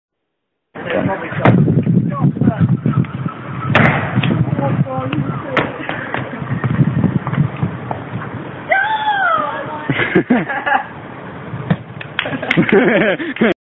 Josh Marshall - City Free-Running
Josh Marshall shows us some of his free-running talents in Norwich City Centre. Not his best though.